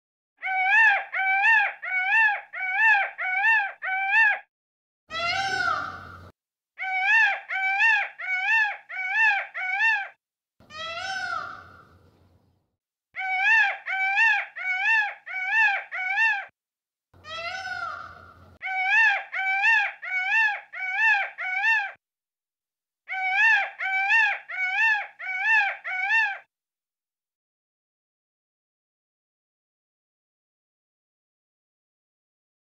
Peacock Sounds 🦚 Peacock Sound Effects
Enjoy these Peacock Sound Effects! Listen to the unique sounds of Peacocks as they call in this video